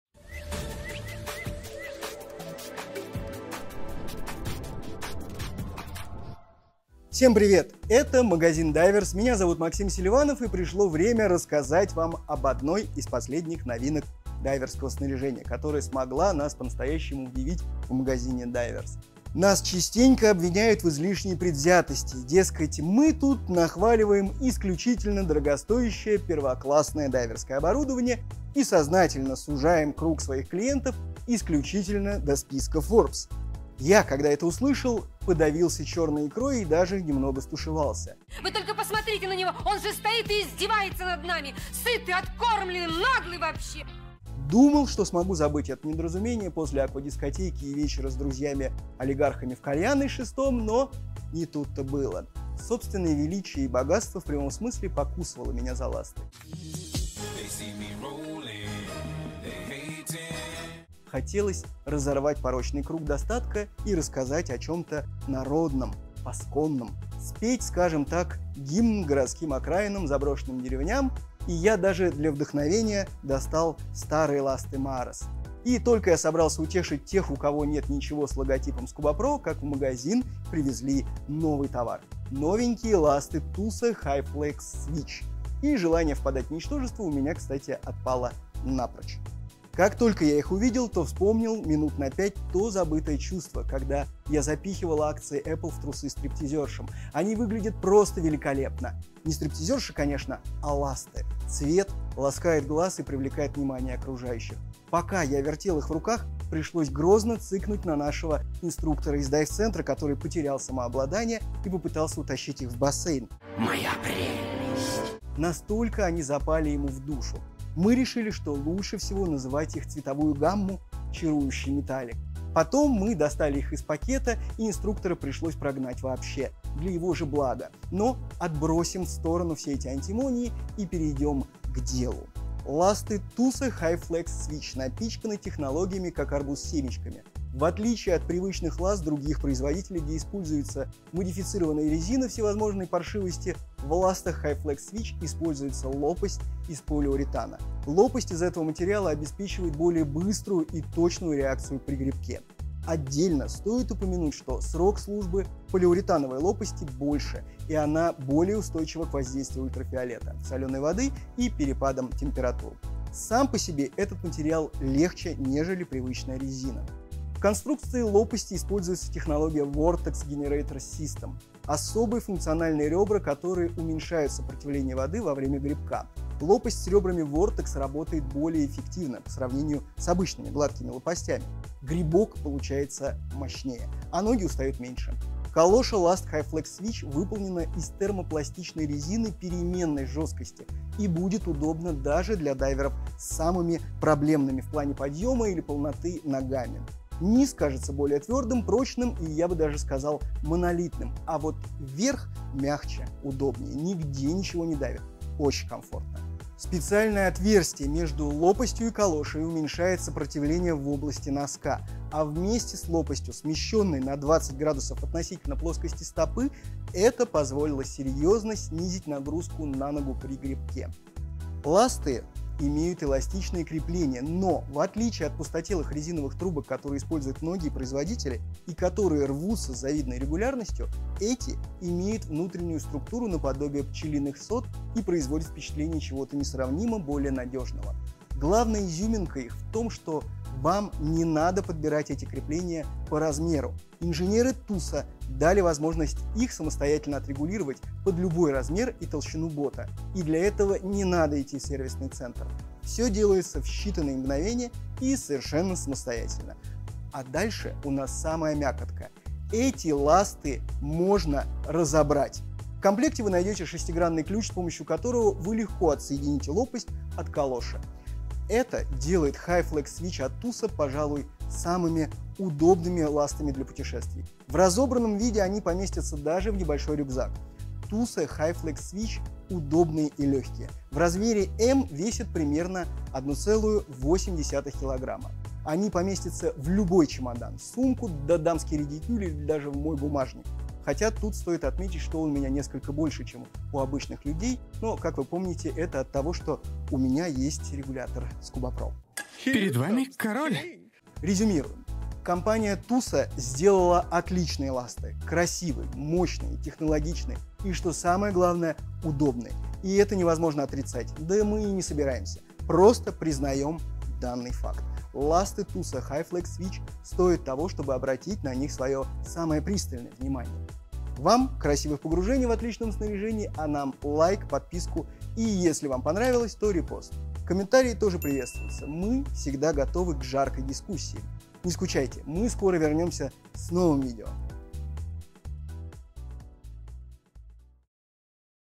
Ласты Tusa Hyflex Switch - собери свою тусу!
* Отдельное спасибо руководителю проекта снаряжения для дайвинга ООО «Импрекс», Гайку Вердяну, который любезно одолжил нам эти замечательные ласты TUSA для теста, восхищения и аплодисментов. Попробуем замахнуться и на подводный контент))

Всем привет! Это магазин Divers, меня зовут Максим Селиванов и пришло время рассказать вам об одной из последних новинок дайверского снаряжения, которая смогла нас по-настоящему удивить.

Нас частенько обвиняют в излишней предвзятости. Дескать, мы тут нахваливаем исключительно дорогостоящее первоклассное дайверское оборудование и сознательно сужаем круг своих клиентов исключительно до списка форбс. Я, когда это услышал, подавился черной икрой и даже немного стушевался. Думал, что смогу забыть это недоразумение после аквадискотеки и вечера с друзьями-олигархами в кальянной с шестом, но не тут то было. Собственное величие и богатство в прямом смысле покусывало меня за ласты. Хотелось разорвать порочный круг достатка и рассказать о чем-то народном, посконном. Спеть, скажем так, гимн городским окраинам, заброшенным деревням. Я даже для вдохновения достал старые ласты Mares. И только я собрался утешить тех, у кого нет ничего с логотипом Scubapro, как в магазин привезли новый товар… новенькие ласты Tusa HyFlex Switch. И желание впадать в ничтожество у меня отпало напрочь.

Как только я их увидел, то вспомнил минут на пять то забытое чувство, когда я запихивал акции Apple в трусы стриптизершам. Они выглядят просто великолепно, не стриптизерши, конечно, а ласты – цвет ласкает глаз и привлекает внимание окружающих. Пока я вертел их в руках, пришлось грозно цыкнуть на нашего инструктора из дайвцентра, который потерял самообладание и попытался утащить их в бассейн – настолько они запали ему в душу! Мы решили, что лучше всего называть их цветовую гамму - «чарующий металлик». Потом мы достали их из пакета и инструктора пришлось прогнать для его же блага. Но отбросим в сторону все эти антимонии и перейдем к делу.

Ласты Tusa HyFlex Switch напичканы технологиями, как арбуз семечками. В отличие от привычных ласт других производителей, где используется модифицированная резина всевозможной паршивости, в ластах HyFlex Switch используется лопасть из полиуретана. Лопасть из этого материала обеспечивает более быструю и точную реакцию при гребке. Отдельно стоит упомянуть, что срок службы полиуретановой лопасти больше и она более устойчива к воздействию ультрафиолета, соленой воды и перепадам температур. Сам по себе этот материал легче, нежели привычная резина.

В конструкции лопасти используется технология Vortex Generator System – особые функциональные ребра, которые уменьшают сопротивление воды во время гребка. Лопасть с ребрами Vortex работает более эффективно по сравнению с обычными гладкими лопастями – гребок получается мощнее, а ноги устают меньше. Калоша ласт HyFlex Switch выполнена из термопластичной резины переменной жесткости и будет удобна даже для дайверов с самыми «проблемными» в плане подъема или полноты ногами. Низ кажется более твердым, прочным. Я бы даже сказал – монолитным, а вот верх мягче, удобнее. Нигде ничего не давит, комфортно.

Специальное отверстие между лопастью и калошей уменьшает сопротивление в области носка. А вместе с лопастью, смещенной на 20 градусов относительно плоскости стопы - это позволило серьезно снизить нагрузку при гребке.

Ласты имеют эластичное крепление. Но в отличие от пустотелых резиновых трубок, которые используют многие производители, и которые рвутся с завидной регулярностью, эти имеют внутреннюю структуру наподобие пчелиных сот и производят впечатление чего-то несравнимо более надежного. Главная изюминка в том, что вам не надо подбирать эти крепления по размеру. Инженеры Tusa дали возможность их самостоятельно отрегулировать под любой размер и толщину бота. И для этого не надо идти в сервисный центр – все делается в считанные мгновения и совершенно самостоятельно. А дальше у нас самая мякотка - эти ласты можно разобрать! В комплекте вы найдете шестигранный ключ, с помощью которого вы легко отсоедините лопасть от калоши.

Это делает HyFlex Switch, пожалуй, самыми удобными ластами для путешествий – в разобранном виде они поместятся даже в небольшой рюкзак. Tusa HyFlex Switch удобные и легкие – в размере М весят примерно 1,8 килограмма. Они поместятся в любой чемодан , сумку, в дамский ридикюль или даже в мой бумажник. Хотя тут стоит отметить, что он у меня несколько больше, чем у обычных людей. Но как вы помните, это от того, что у меня есть регулятор Scubapro.

Резюмируем. Компания Tusa сделала отличные ласты. Красивые, мощные, технологичные и, что самое главное, удобные. И это невозможно отрицать. Да мы и не собираемся. Просто признаем данный факт.  Ласты Tusa HyFlex Switch стоят того, чтобы обратить на них свое внимание.

Комментарии тоже приветствуются, мы всегда готовы к жаркой дискуссии. Не скучайте, мы скоро вернемся с новым видео!